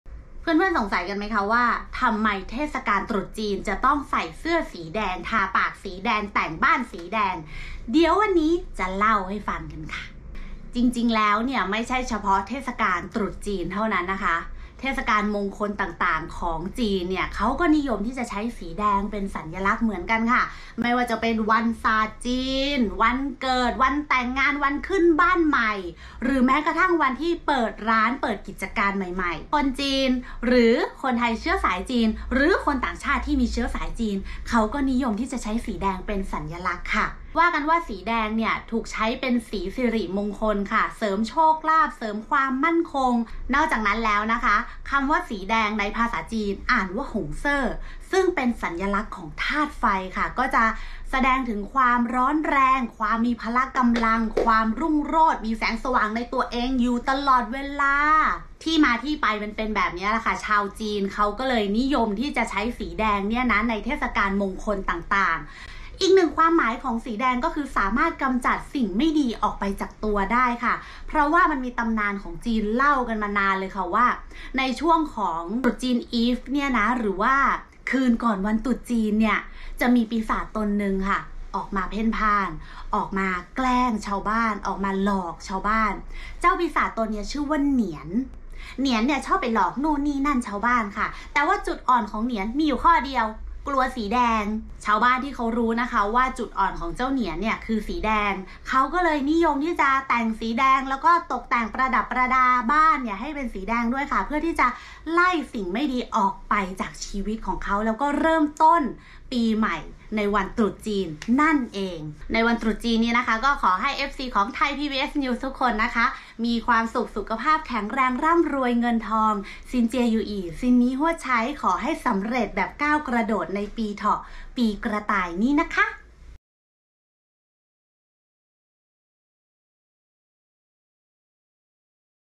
ทำไม “ตรุษจีน” ต้องใส่สีแดง | ข่าวเล่าเรื่อง
ใครๆ ก็รู้ว่าสีแดงเป็นสีมงคลของจีน แล้วมีที่มาที่ไปอย่างไร ข่าวเล่าเรื่อง โดย ปวริศา บุญเรือง